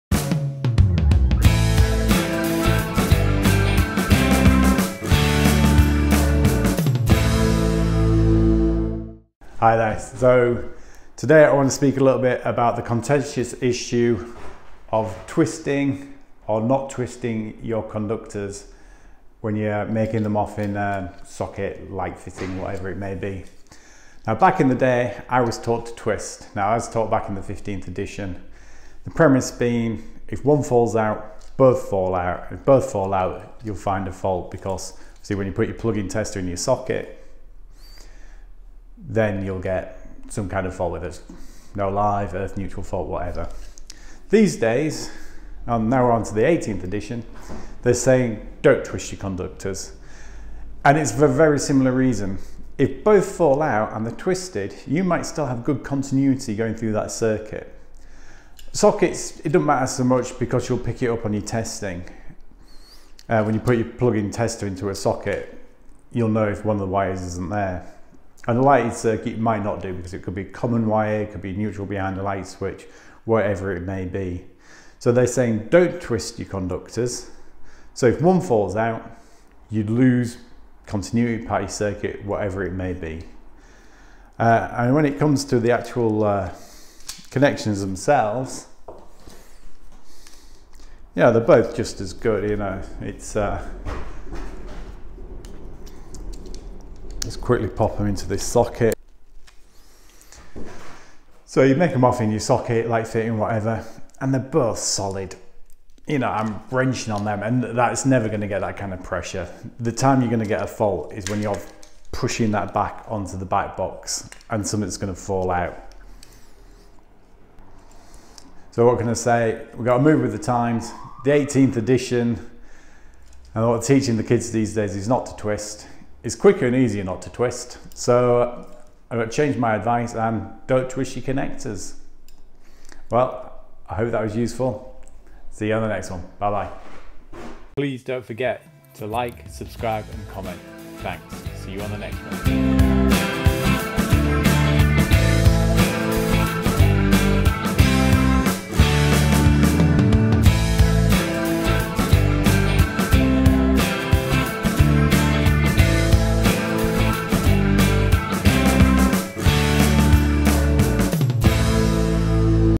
Cable ends; to twist or not to twist
As the IET regs evolve so does the training and what is regarded best practice so as an update to my advice on previous videos; Should you twist or not twist the cable ends in terminals?

Spoiler Alert... No...

But watch the video too for a proper and full explanation of what is the IET 18th Edition Best practice for electricians.